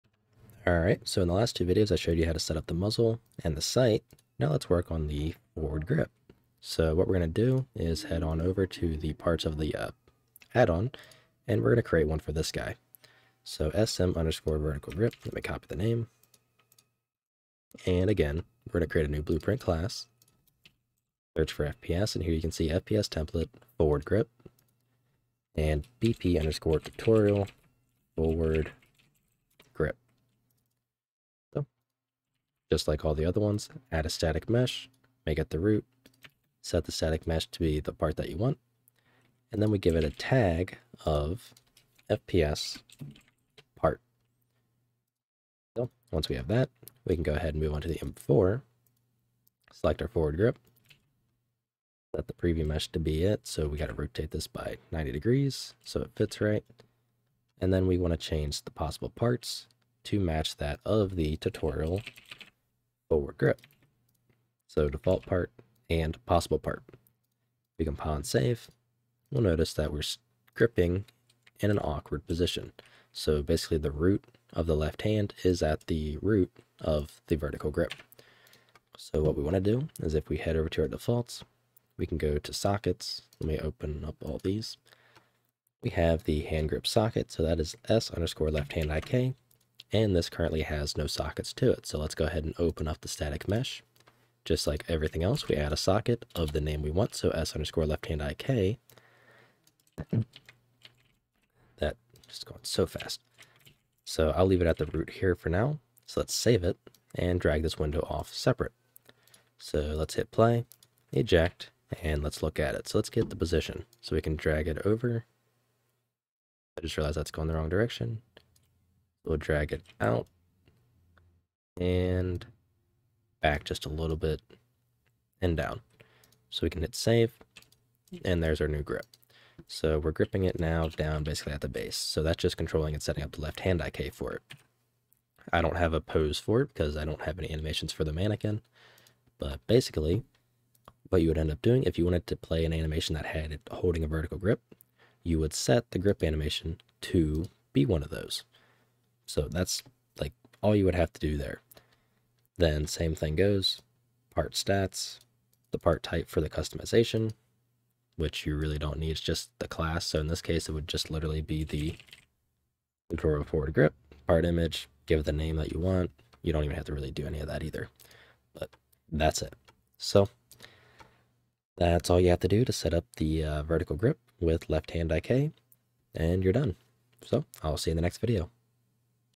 UMFPSTemplate Tutorial: "Setup New Forward Grip"
In this video I cover how to setup a new forward grip.